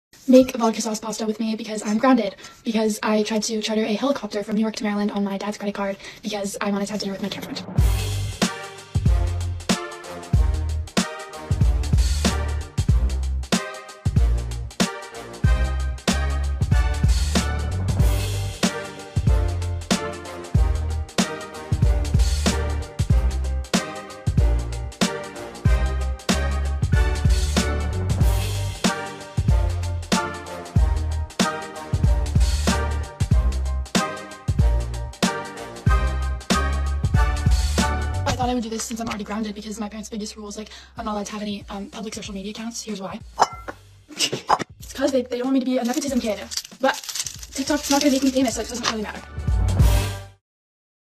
Sofia Coppola’s daughter Romy was grounded for trying to charter a helicopter | Page Six
The eldest daughter of acclaimed filmmaker Sofia Coppola, 16-year-old Romy, just revealed in a totally unrelatable yet hilarious TikTok that she was grounded for attempting to rent a chopper using her musician father Thomas Mars’ credit card.

“Make a vodka-sauce pasta with me because I’m grounded because I tried to charter a helicopter from New York to Maryland on my dad’s credit card because I wanted to have dinner with a camp friend,” Romy began the video, which only got funnier.

The teen went on to disclose that she did not “know the difference between a garlic and an onion,” so she “had to Google images of onions on [her] phone.”

She claimed to be “embarrassed” by the admission.

“Also, I thought I would do this since I’m already grounded since my parents’ biggest rule is, like, I’m not allowed to have any public social media accounts,” she said of joining the platform.